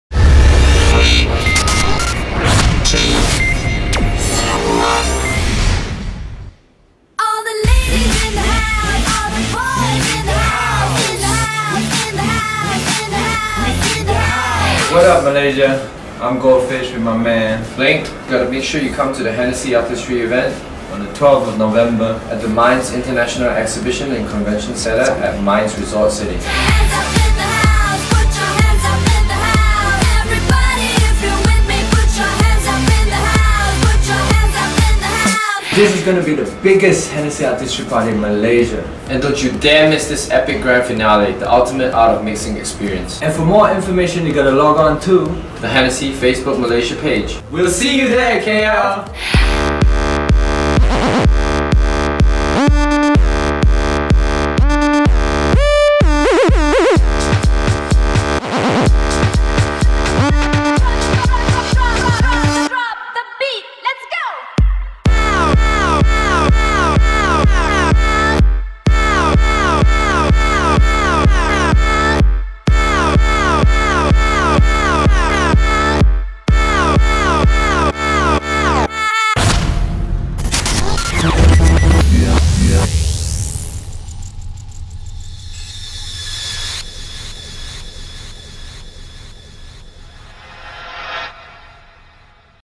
Hennessy Artistry 2011 presents Goldfish & Blink!
'The Global Art of Mixing Party' is here!
Join us at the Hennessy Artistry party happening on 12.11.11 at Malaysia International Exhibition & Convention Centre (MIECC) at Mines Resort City.

GOLDFISH & BLINK
KL's Superstar DJs famous for their energetic live performances and their mix of music genres, ranging from break beats, hip hop, house to electro. The powerhouse DJs, is set to blow the night away!

Join our Hennessy Malaysia Facebook page and find out how you can win exclusive invites to the 'The Global Art of Mixing Party'!